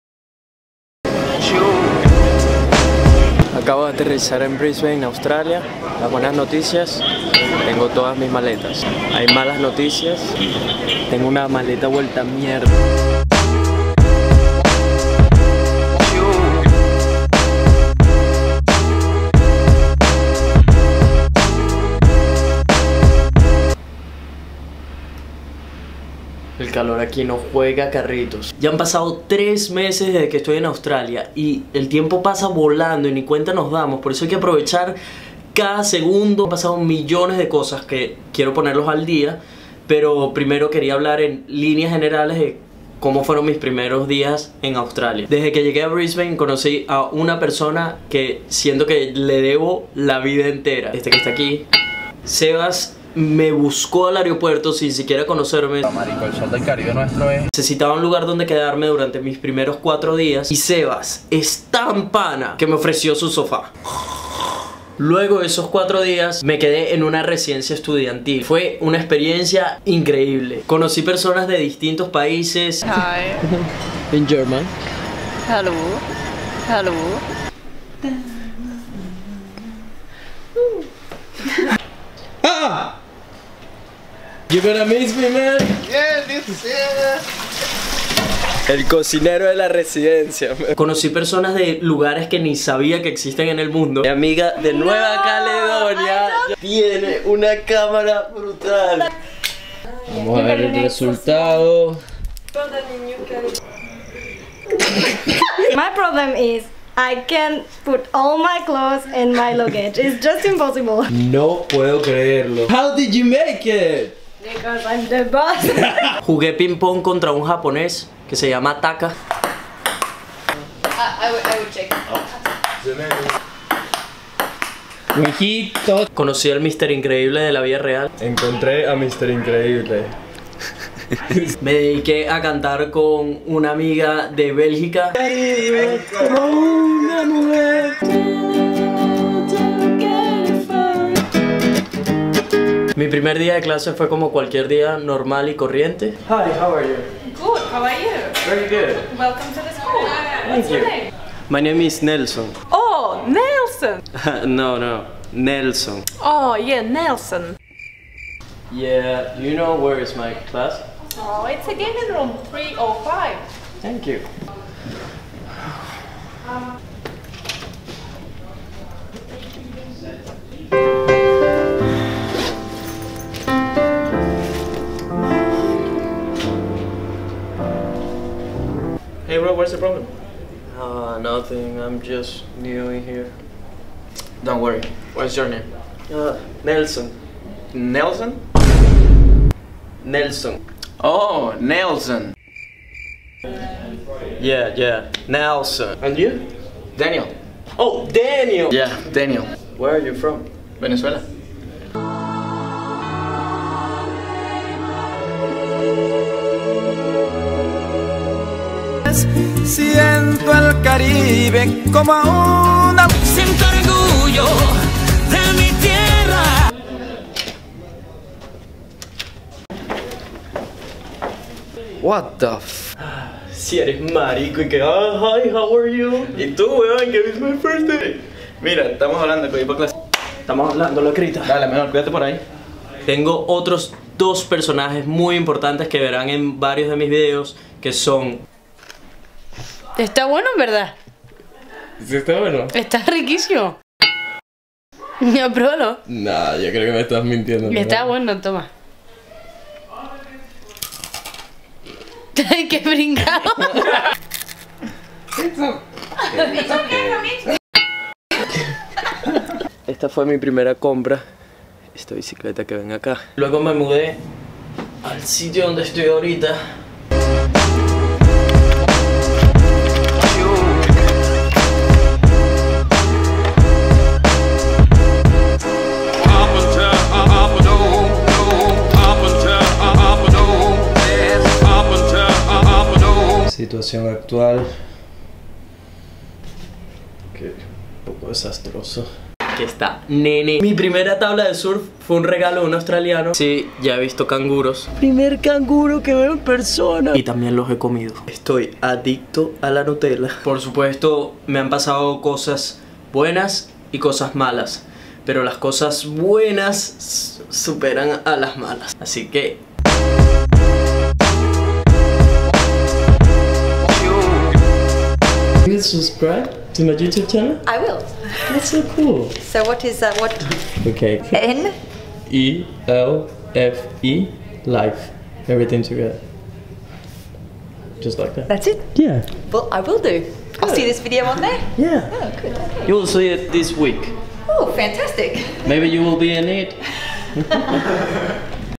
MIS PRIMEROS DIAS EN AUSTRALIA | MY FIRST DAYS IN AUSTRALIA - nelfelife Ep 1
Gente buena vibra ya tengo 3 meses en AUSTRALIA! aqui les cuento en lineas generales como fueron mis PRIMEROS DIAS! muchas experiencias buenas y por supuesto malas también, ya les contare mas adelante. Las mejores vibras para todos!